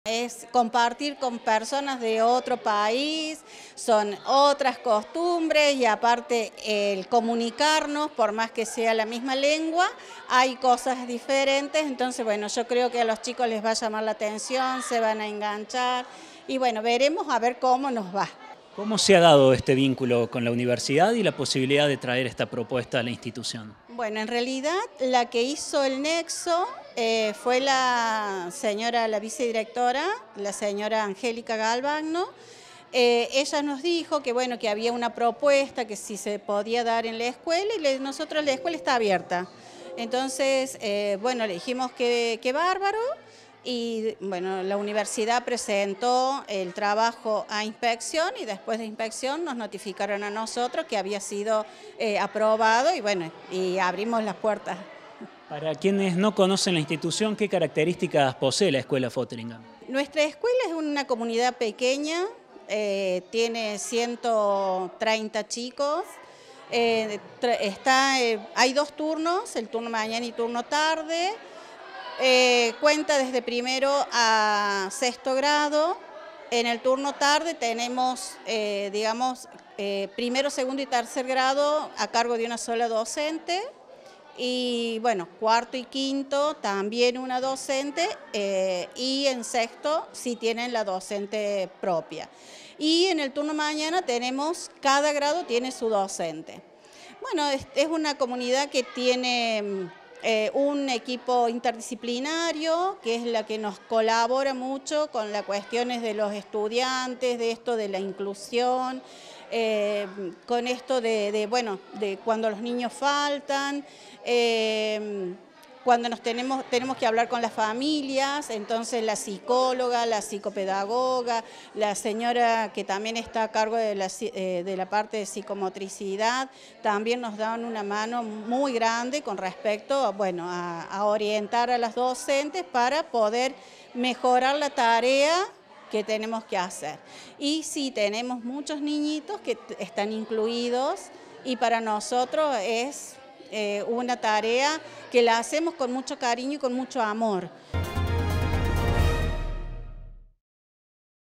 SUSANA ÁLVAREZ – DIRECTORA ESCUELA GRAL. FOTHERINGHAM
NUEVO CAFÉ CIENTÍFICO CON PROYECCIÓN DE PELÍCULA Y REFLEXIÓN EN LA ESCUELA FOTHERINGHAM
UniRio TV
Universidad Nacional de Río Cuarto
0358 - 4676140